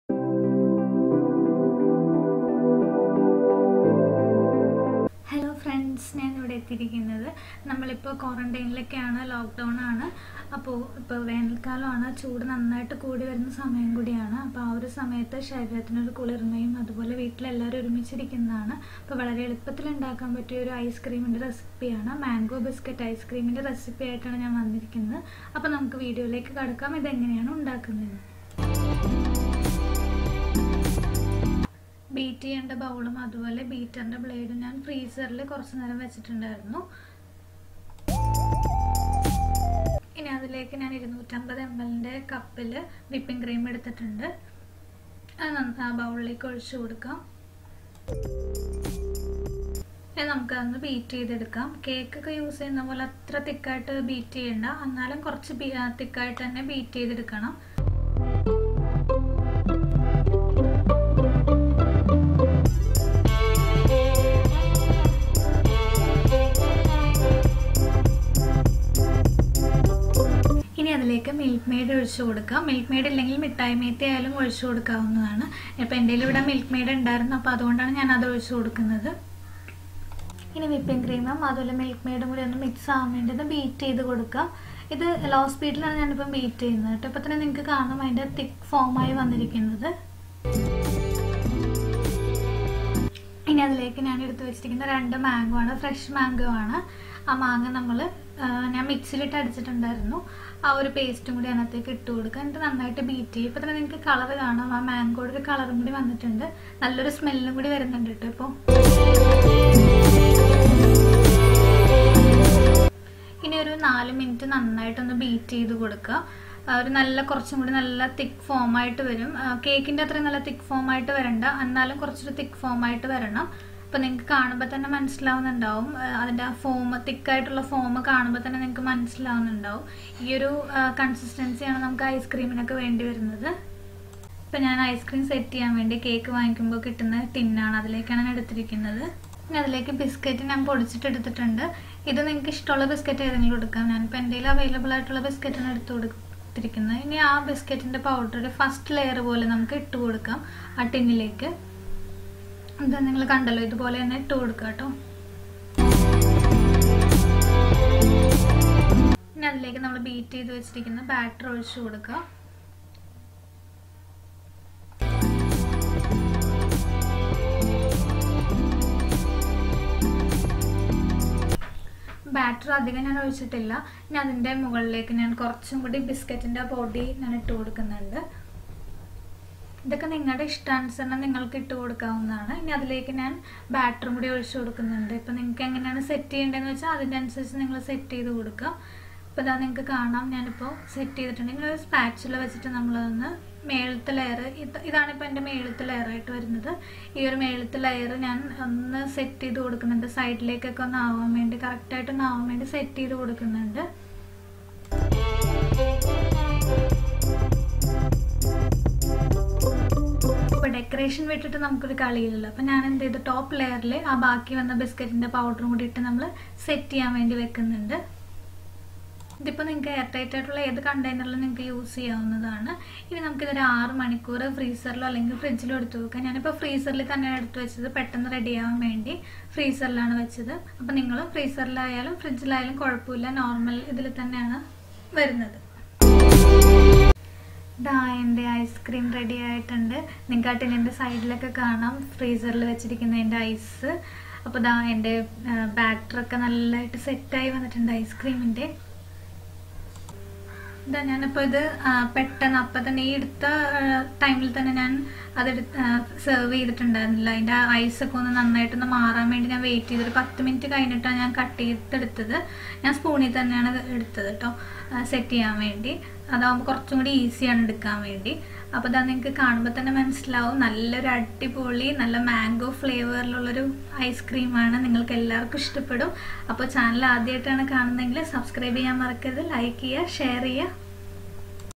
ഈ ചൂടത്ത് ഒരു ഐസ്ക്രീം ആയാലോ! | How to make easy MANGO BISCUIT ഐസ്ക്രീം | at Home | AnsiShemi Corner
Today I 'm coming Mango Biscuit ഐസ്ക്രീം I hope you will like this.

Preparation: Please follow the instructions shown in the video.

AnsiShemi Corner